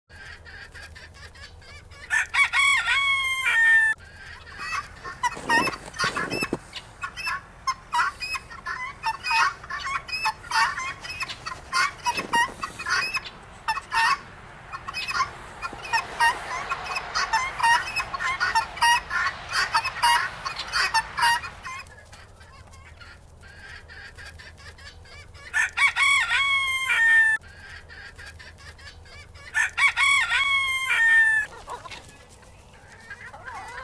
The Guinea Hen Trio
This trio of Guinea hens found a nice perch on our old grinder wheel. They were enjoying this brisk day and voicing their opinion about Guinea life in general. Guinea fowl are great bug eaters and watch dogs.